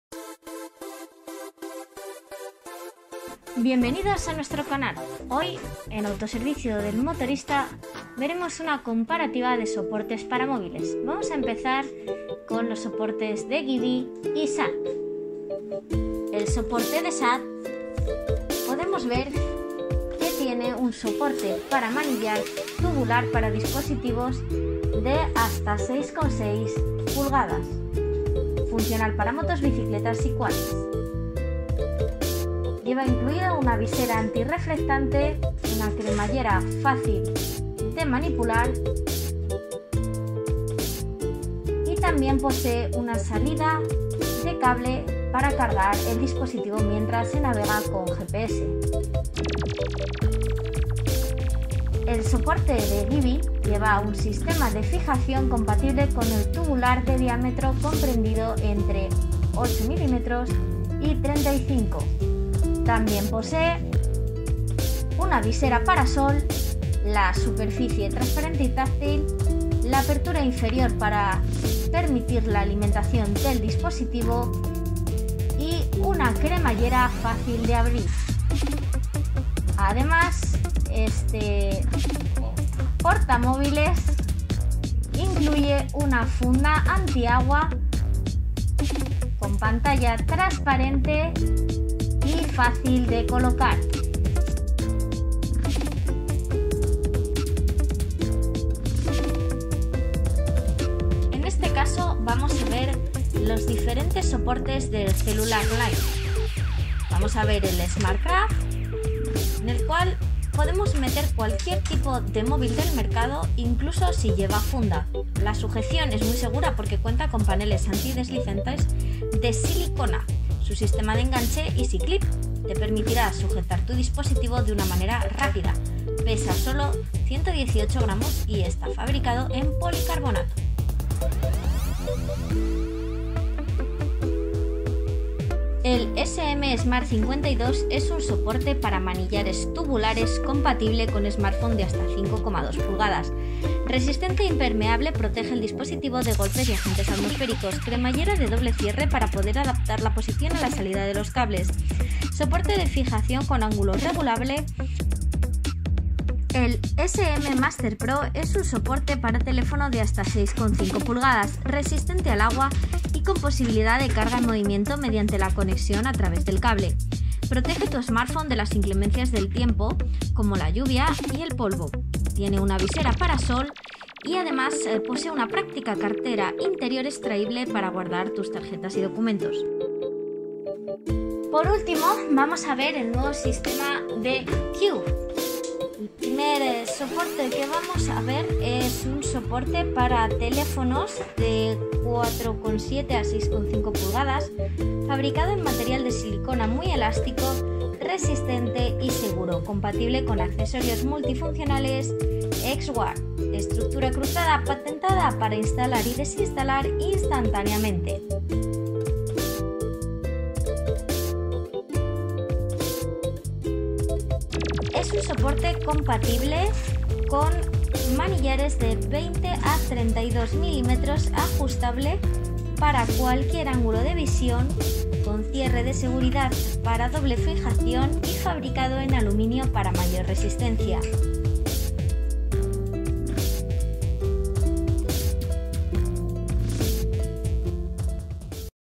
Comparativa soporte para móviles
Os traemos una comparativa de los diferentes soportes para móviles que tenemos en nuestras tiendas.